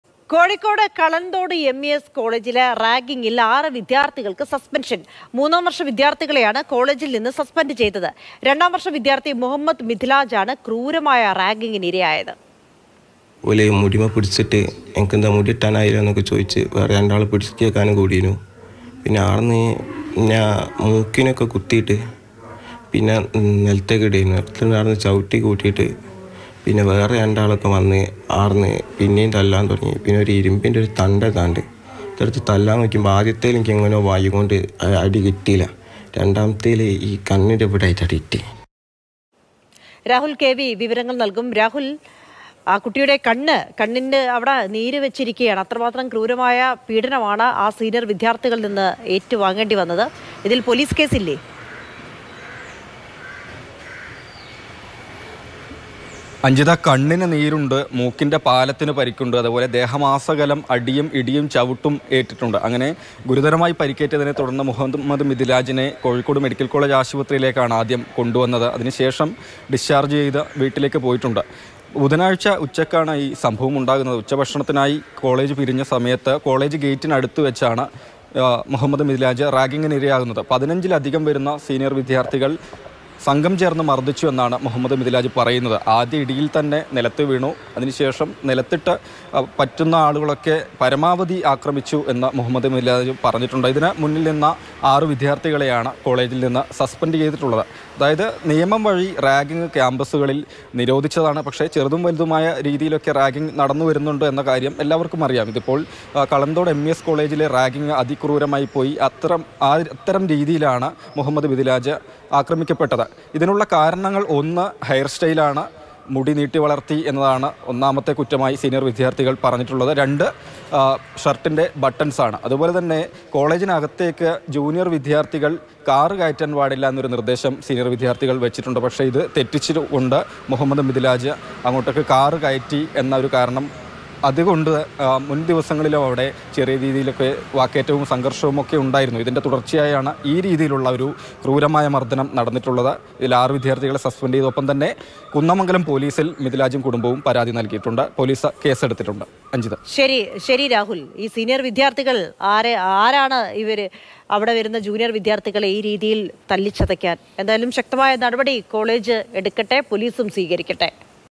കോഴിക്കോട് റാഗിങിന്റെ പേരില്‍ വിദ്യാര്‍ത്ഥിക്ക് ക്രൂരമര്‍ദ്ദനം; കാഴ്ചയ്ക്ക് തകരാര്‍ | Ragging
മൂന്നാം വർഷ  വിദ്യാർഥികളെയാണ് കോളേജിൽ നിന്ന് സസ്‌പെൻഡ് ചെയ്തത്. രണ്ടാം വർഷ വിദ്യാർഥിയാണ് ക്രൂരമർദനത്തിന് ഇരയായത്.

Happy viewing!